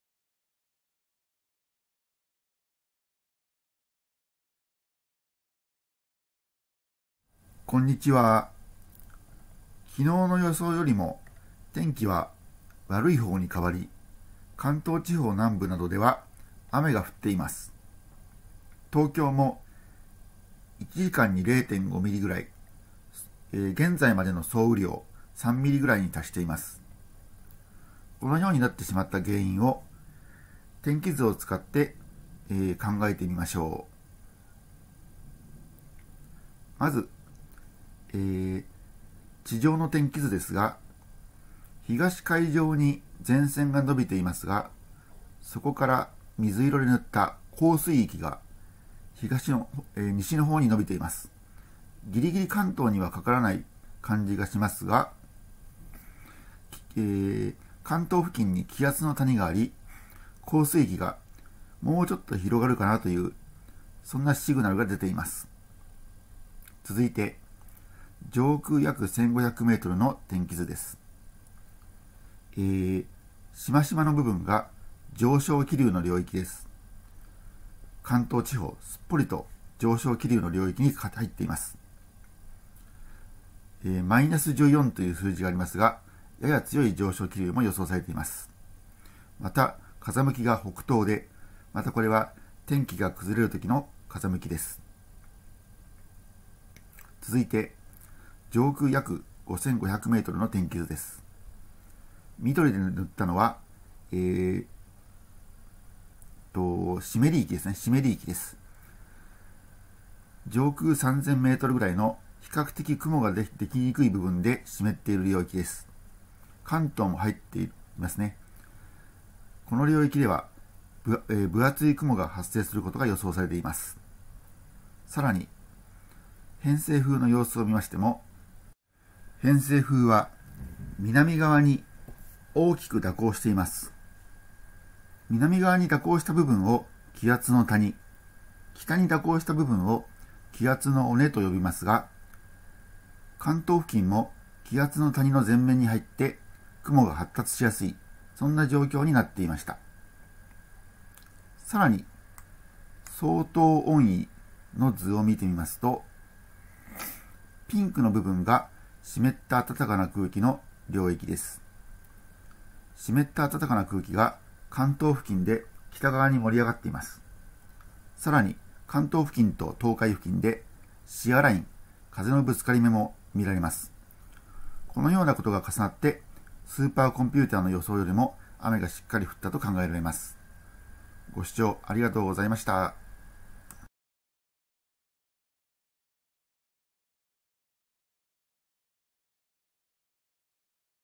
【2023年3月28日】関東地方で意外にしっかりと雨が降る
曇り予報だった関東地方でも、意外にしっかりと雨が降ってしまいました。天気図で見ると、その様子がよくわかります。
※気象庁データ使用。